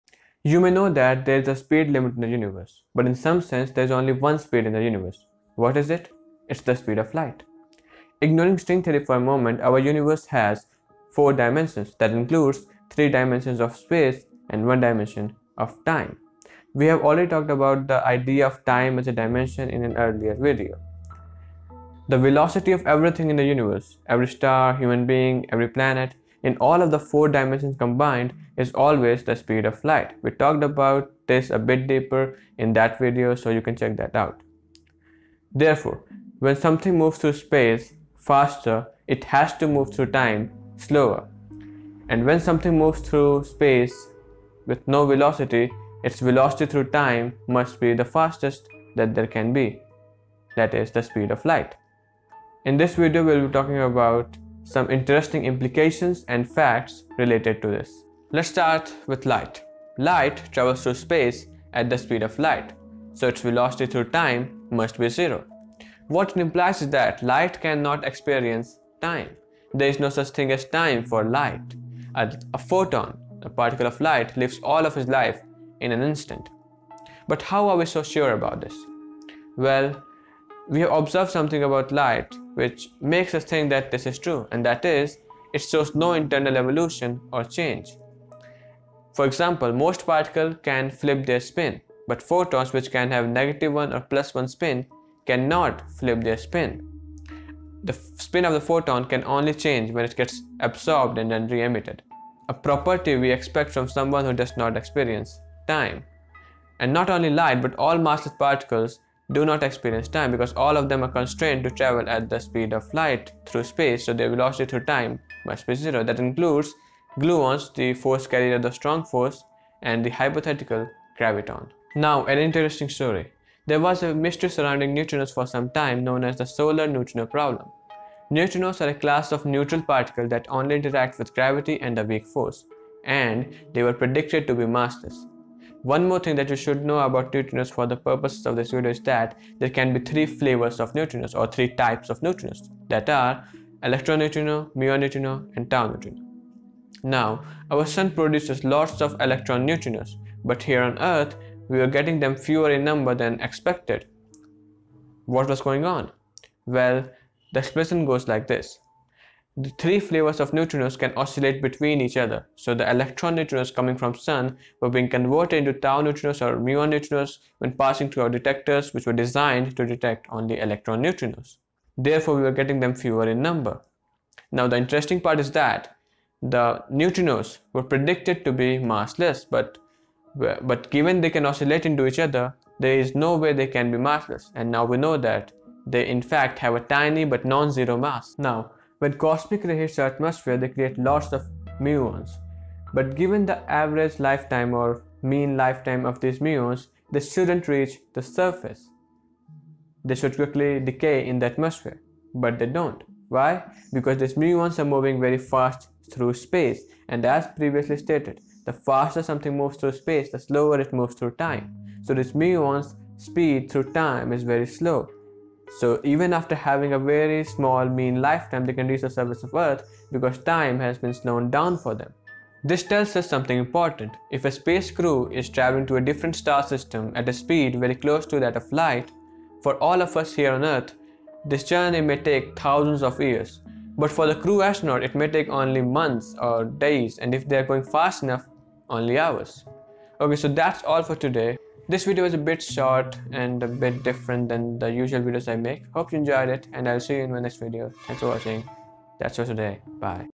PHOTONS do not experience TIME.
In this video we are talking about three different phenomena , which shows us the weird effects of time. First , since photons travels at the speed of light they do not experience time. Thus spin of photons never flips. Second , Neutrinos were first predicted to be massless. But we have observed neutrino oscillations , since neutrinos can't oscillate without experiencing time , they can't be massless. As massless particles cannot experience time.
Third , when cosmic ray hit our atmosphere they create lots of muons , but given the average lifetime of these muons they shouldn't reach the surface , yet they do because these muons are moving very fast so time runs slower for them and they reach the surface.

time stamps :
00:00 Introduction
00:58 Photons do not experience time
02:01 neutrinos do experience time , therefore...
03:08 effects of time dilation on muons

Written , Presented and Edited by :
PRIYANSH